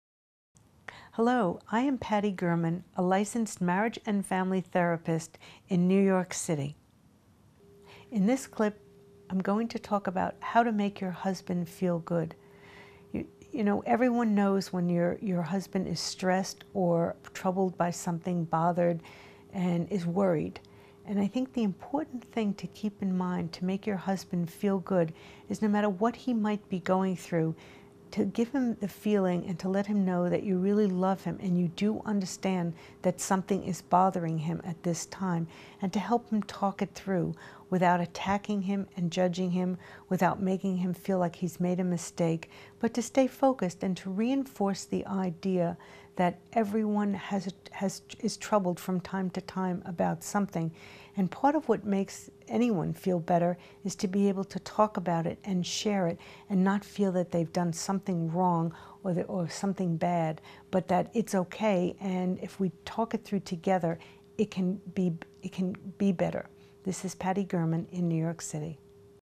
Interpersonal Relationship Advice : How to Make Your Husband Feel Good
Making your husband feel good starts with understanding his situation and what is bothering him. You can support him by helping him talk through whatever it is with advice from a licensed marriage and family therapist in this free video on relationships and romance.

Bio: Patti German is a member of the American Association for Marriage and Family Therapy. She has certification in marriage and family therapy (MFT) and has worked with the Penn Council for Relationships.